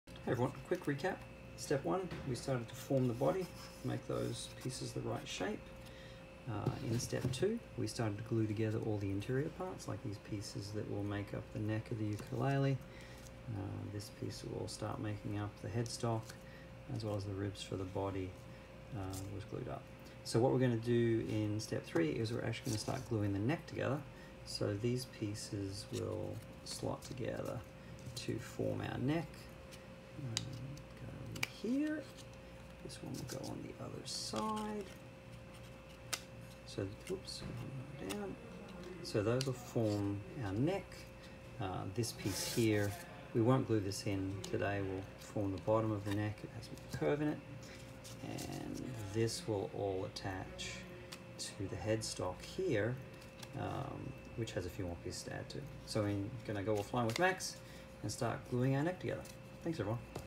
Ukulele Build - Step 3 - Neck
Ukulele build from scratch with Max.  Step 3 - glue up neck parts.

Want to build your own ukulele from laser cut parts?  You can purchase my soprano ukulele design files on Etsy